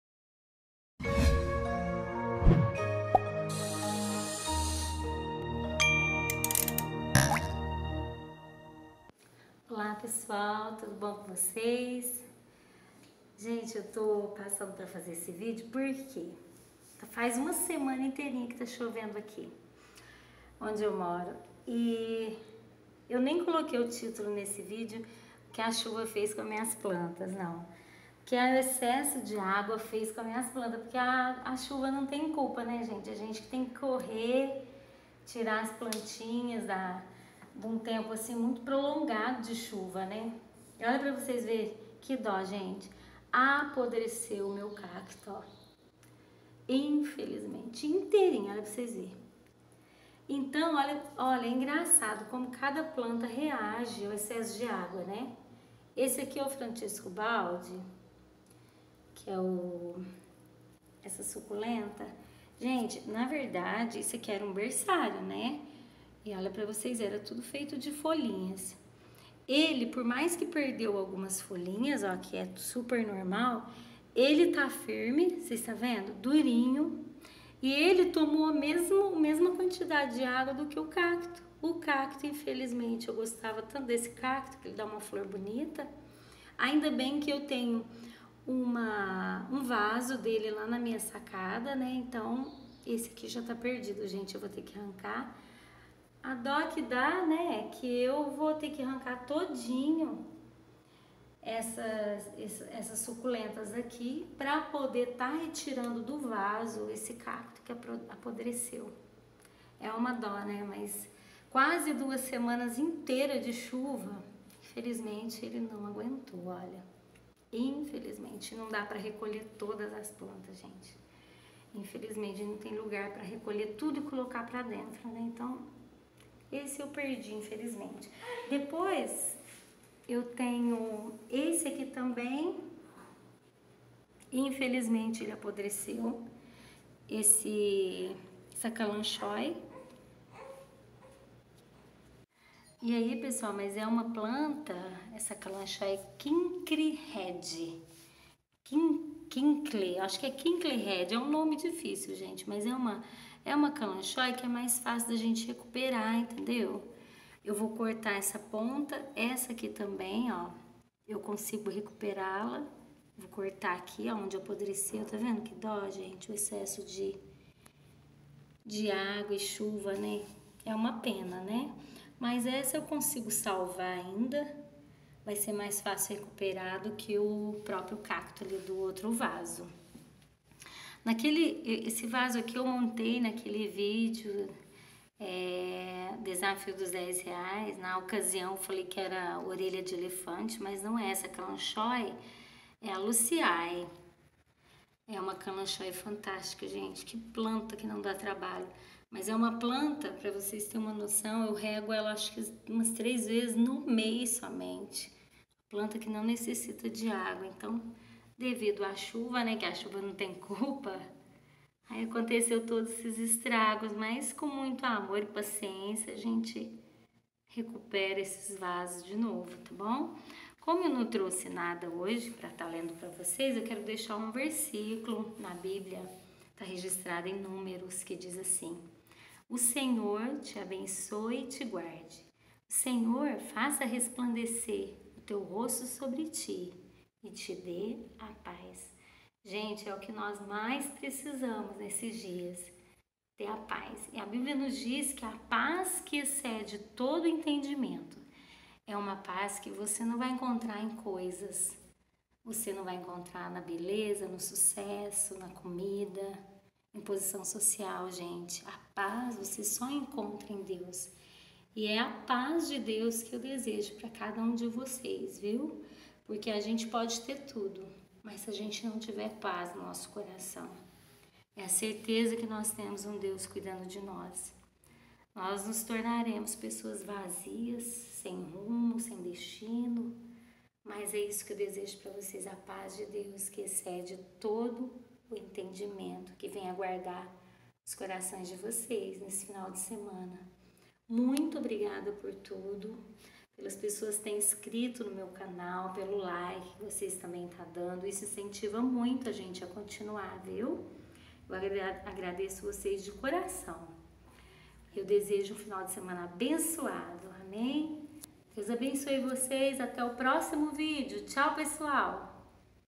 O QUE O EXCESSO DE ÁGUA FEZ COM AS MINHAS PLANTAS
Olá pessoal, nesse vídeo venho mostrar o que aconteceu com algumas das minhas plantas devido muita chuva aqui na minha cidade.
Espero que gostem, dê um like para ajudar o Canal...